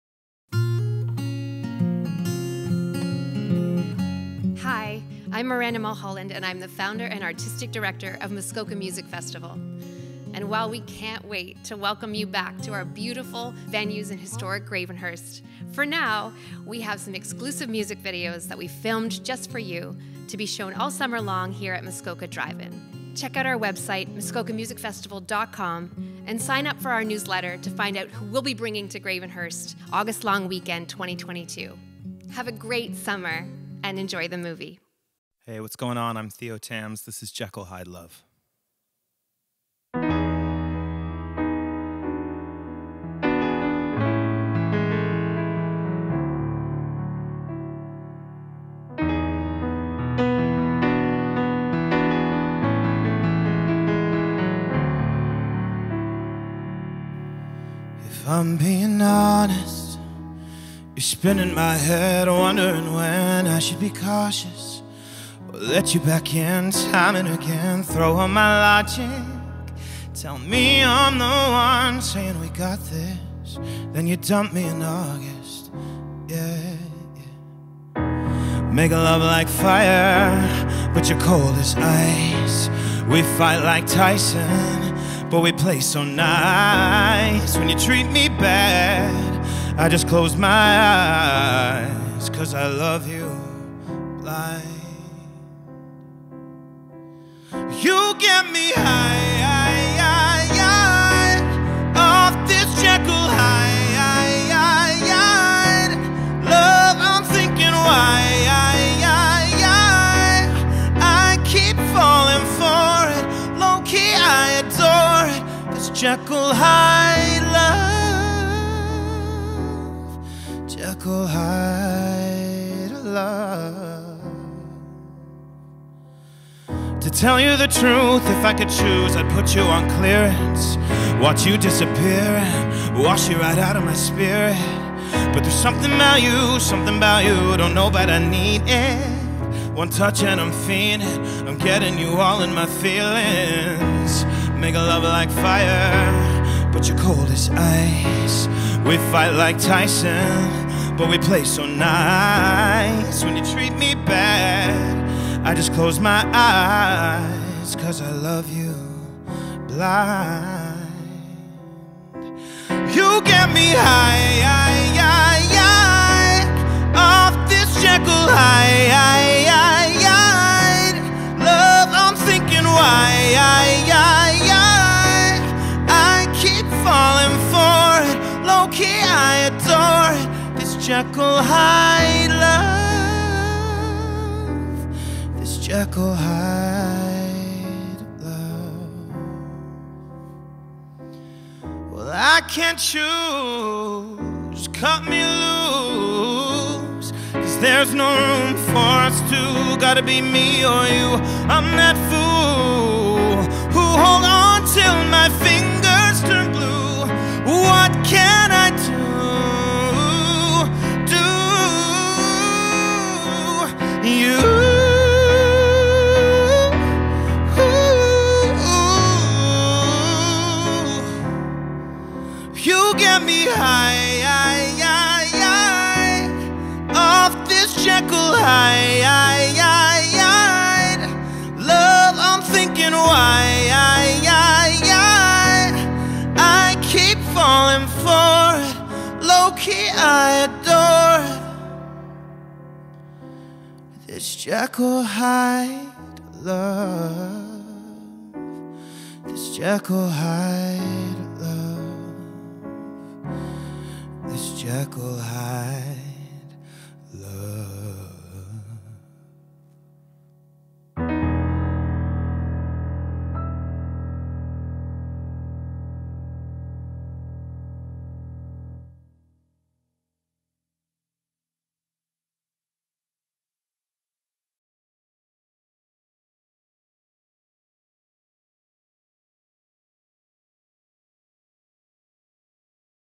Theo Tams for Muskoka Music Festival
While we can't be with you as we'd planned again this year in all our wonderful venues in downtown Gravenhurst we came up with something fun. We commissioned songs from some amazing Canadian artists just for you. They will be shown before the movies at the Muskoka Drive In! Go to our website and sign up for our newsletter to be the first to find out who we'll be bringing to the Muskoka Music Festival over the August long weekend in 2022.